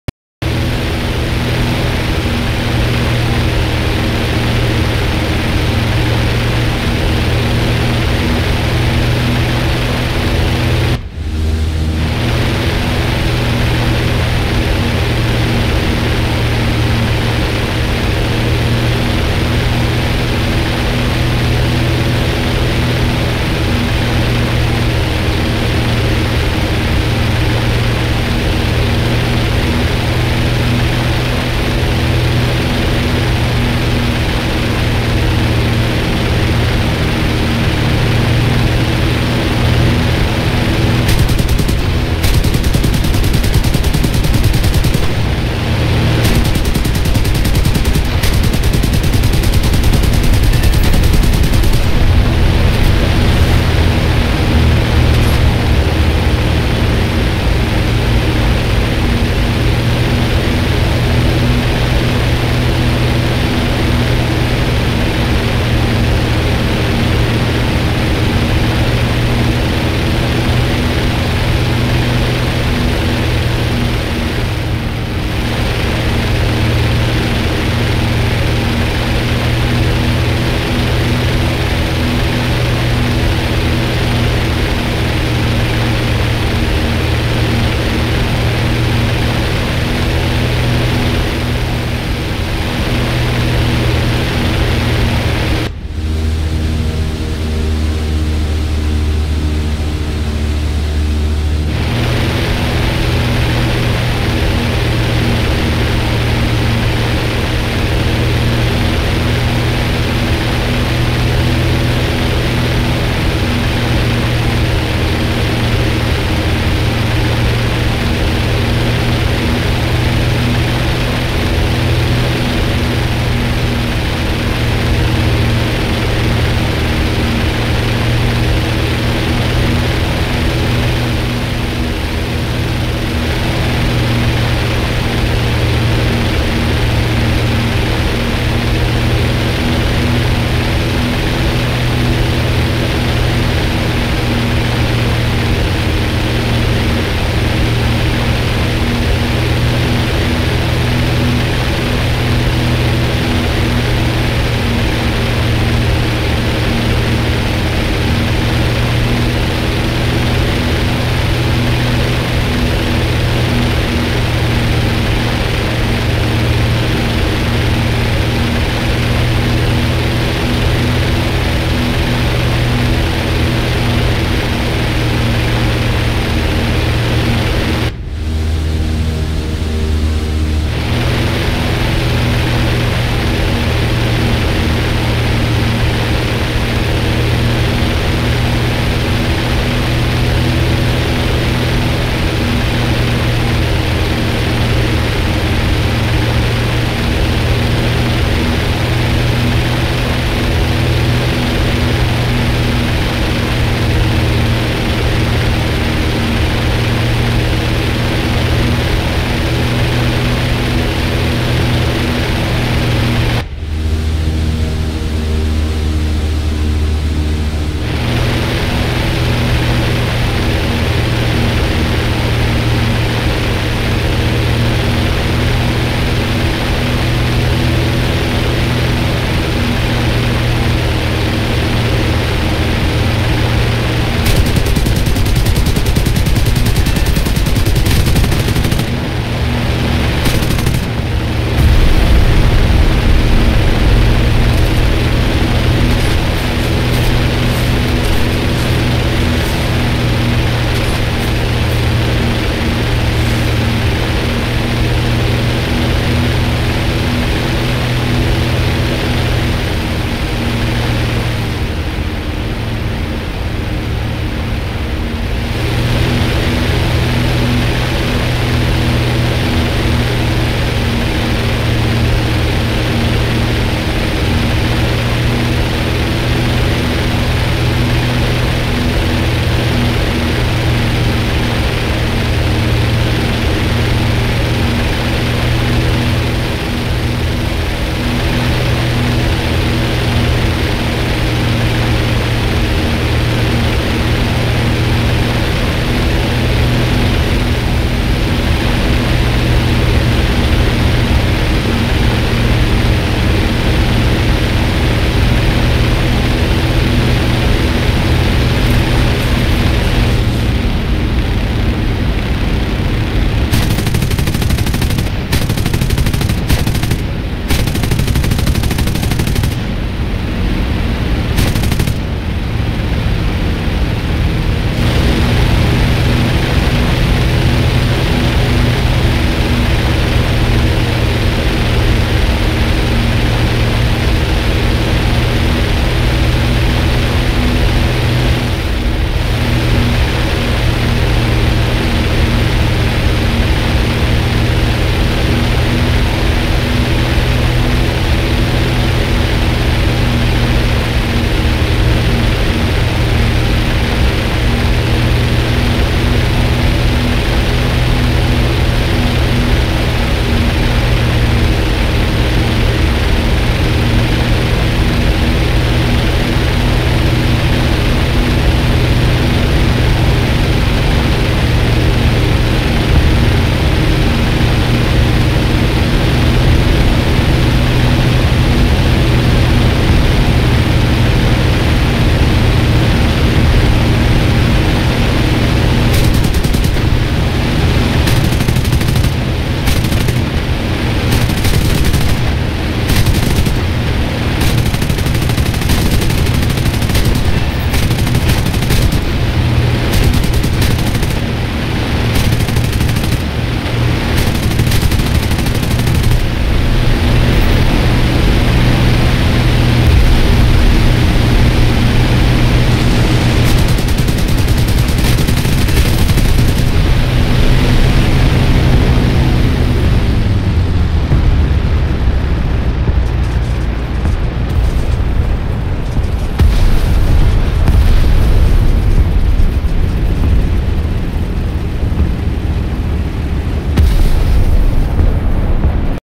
20190507-225920 AF-RB-US 4-0-0-0_1w P-61A-1
My double record 4 kills and 2 fighters. The BV 238 died a while after me.